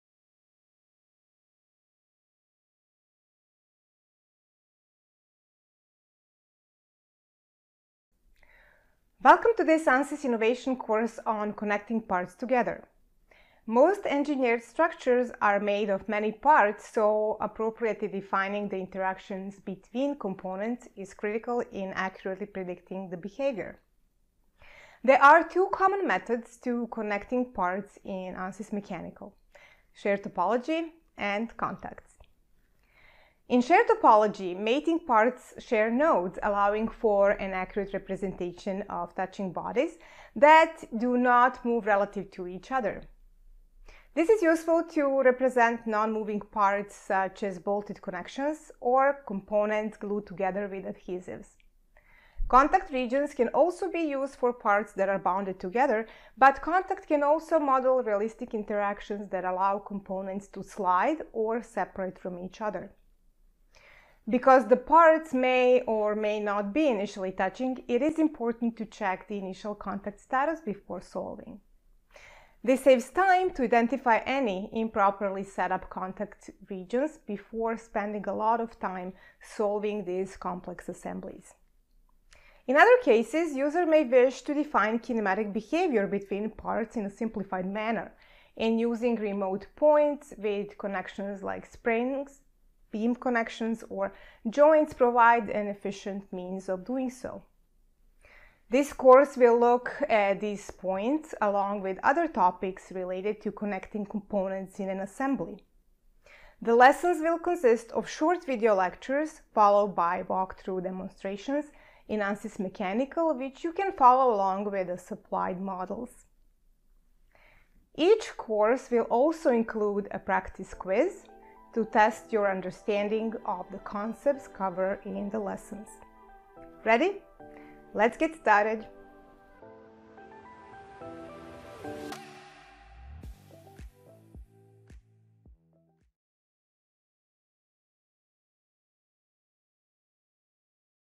Connecting Parts Together Using Ansys Mechanical – Course Overview
While analyzing engineering structures that are made up of many parts, it is extremely important to appropriately define the interactions between components. In this course, you will learn the two common methods for connecting parts in Ansys Mechanical – shared topology and contacts. Understanding the basics of contacts will help in modeling realistic interactions between parts like sliding or separation, whereas checking initial contact conditions will help to avoid failures due to improper contacts. This course will also focus on the use of remote points for defining kinematic behavior between parts using connections like springs, beam connections and joints in a simplified manner.